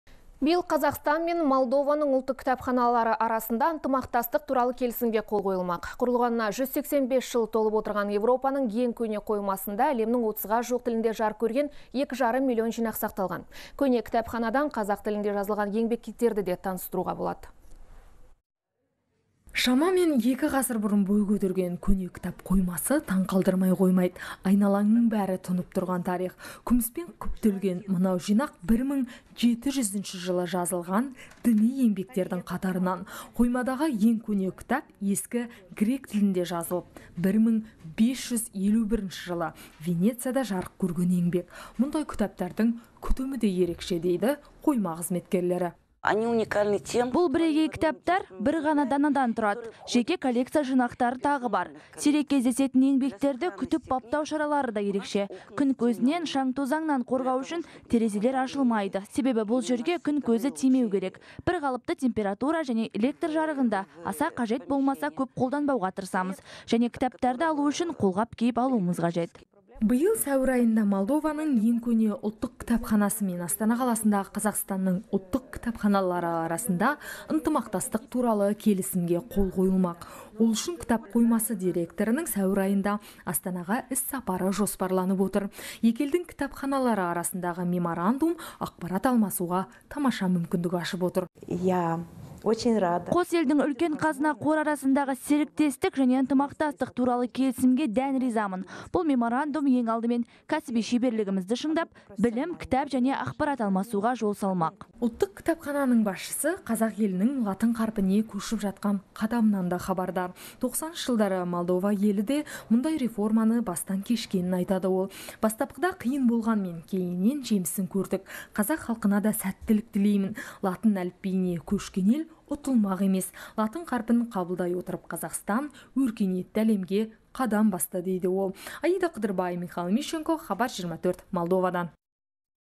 Қазақстан мен Молдованың ұлттық кітапханалары ынтымақтастық келісіміне қол қоймақ
Биыл Қазақстан мен Молдованың ұлттық кітапханалары арасында ынтымақтастық туралы келісімге қол қойылмақ.

Құрылғанына 185 жыл толып отырған Еуропаның ең көне қоймасында әлемнің 30-ға жуық тілінде жарық көрген 2,5 млн жинақ сақталған. Көне кітапханадан қазақ тілінде жазылған еңбектермен де танысуға болады.

Шамамен 2 ғасыр бұрын бой көтерген көне кітап қоймасы таңғалдырмай қоймайды. Айналаңның бәрі тұнып тұрған тарих. Күміспен күптелген жинақ 1700 жылы жазылған діни еңбектердің қатарынан. Қоймадағы ең көне кітап – ескі грек тілінде жазылып, 1551 жылы Венецияда жарық көрген еңбек. «Мұндай кітаптардың күтімі де ерекше», – дейді қойма қызметкерлері.